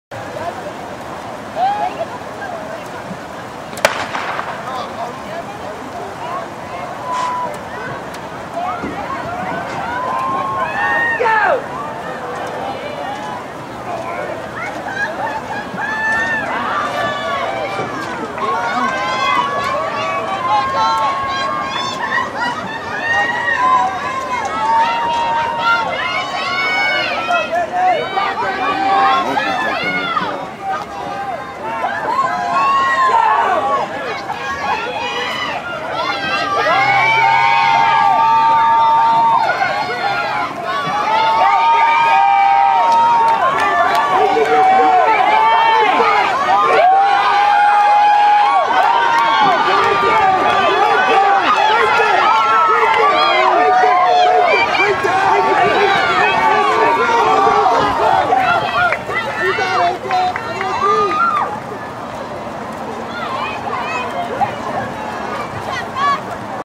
O'Dea High School Track & Field 2010: Meet #1, 400M, Heat 1
Chris Kidd from of O'dea High School rolls in the 400 meters at Ingram
High School. (3/25/2010)